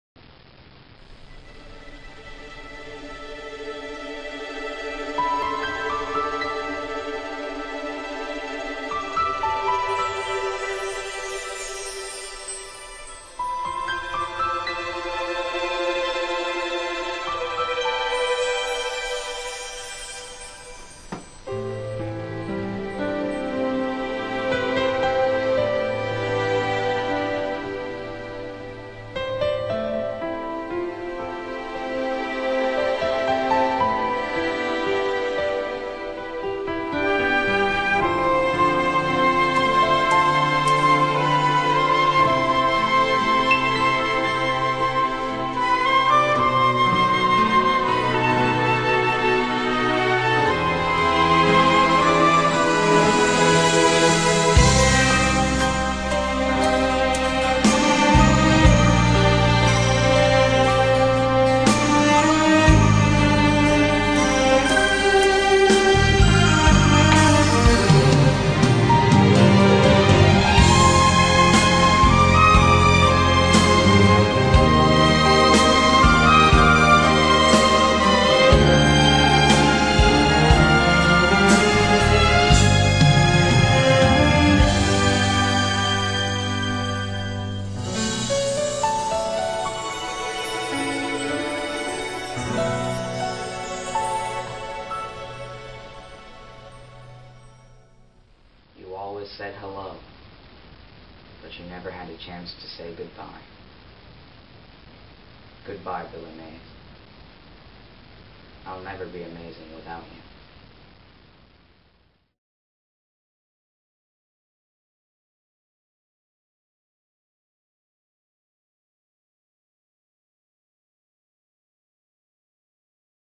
In Memory of Billy Mays~A True Commercial Hero
I can never thank Billy Mays enough for the both the laughs and the insight he was provided me.

I will forever mourn his passing.